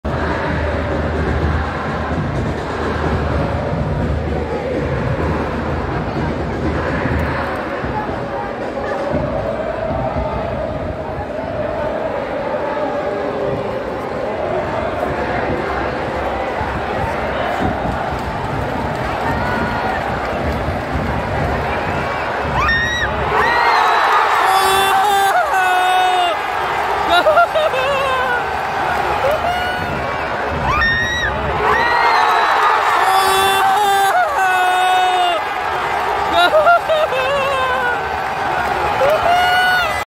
GOOLLL..Tendangan Bebas Rizky Dwi Ke Gawang PSIS||Leg Kedua Semifinal Piala Presiden 2022
GOOLLL..Tendangan Bebas Risky Dwi Ke Gawang PSIS||Leg Kedua Semifinal Piala Presiden 2022